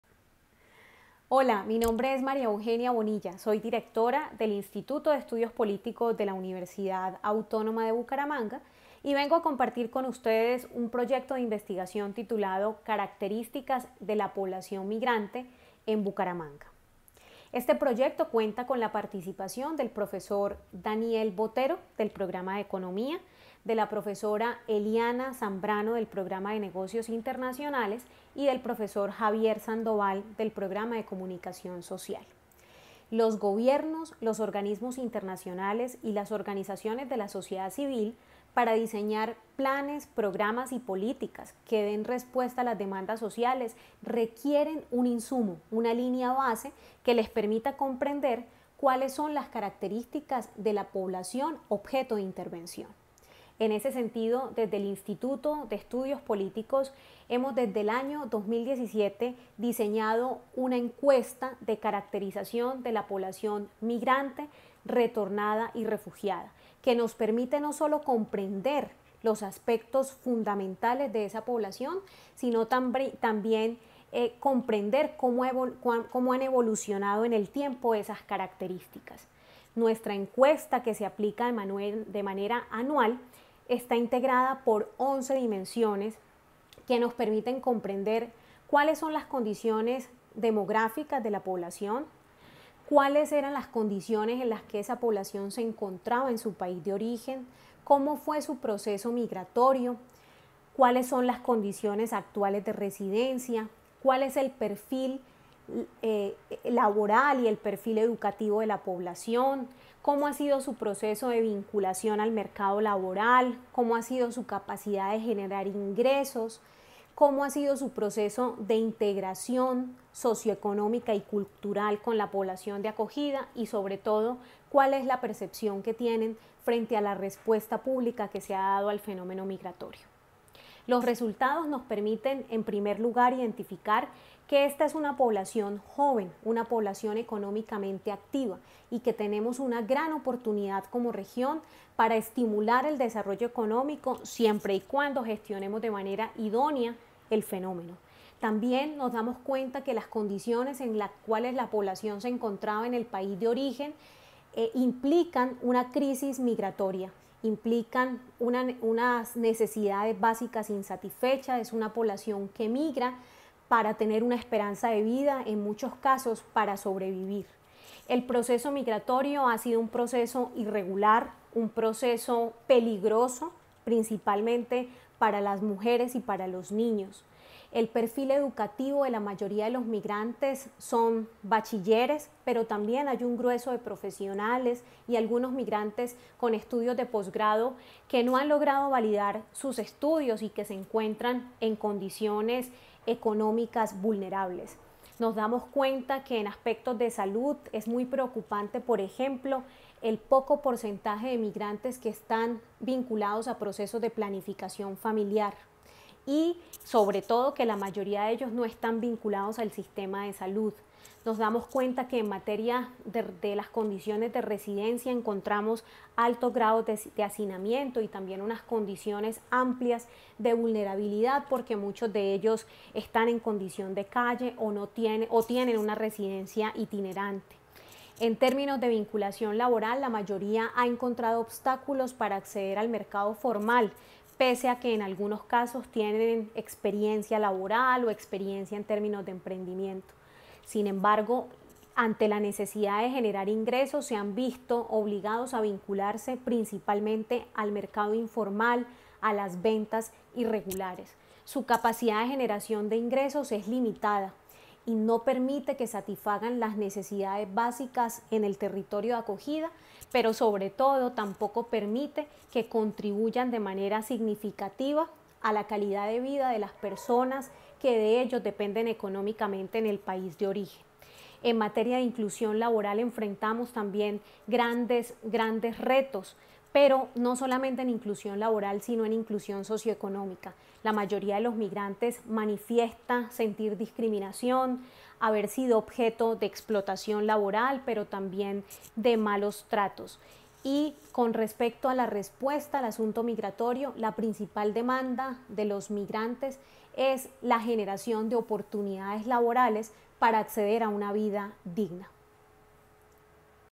Analisis comparado de las caracteristicas sociodemograficas de la poblacion migrante proveniente
Analisis comparado de las caracteristicas sociodemograficas de la poblacion migrante proveniente de Venezuela radicada en Bogotá y Bucaramanga